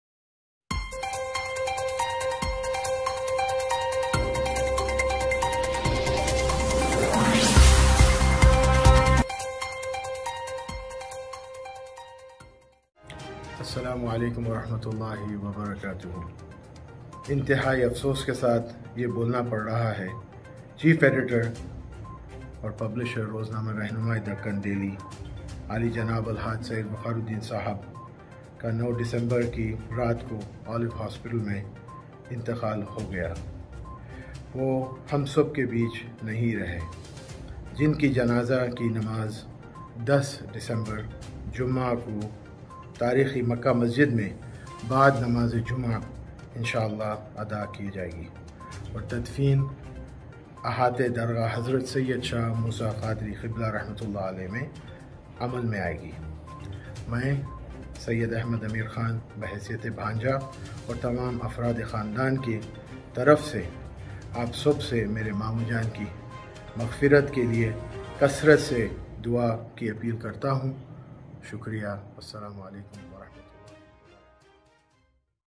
Chief Editor Rahnuma E Deccan Urdu Daily Janab Syed Viqar Uddin Quadri Sahab Is No More
Chief Editor Rahnuma E Deccan Urdu Daily Janab Syed Viqar Uddin Quadri Sahab Ka Inteeqal .
Marhum Ki Namaz E Janaza 10Th December Friday Ko Bad Namaz E Jumma Macca Masjid Mei Aada Ki Jaiyeegi .
Aur Tadfeen Dargah Hazrath Syed Shah Moosa Quadri Qibla RH Mei Amal Mei Aayeegi .
Marhum Ke Bhanjee Syed Ahmed Ameer Khan Ne Sabhi See InKe Mamu_Jaan Ki Magfeerat Ke Leye Dua Ki Appeal Ki Hai .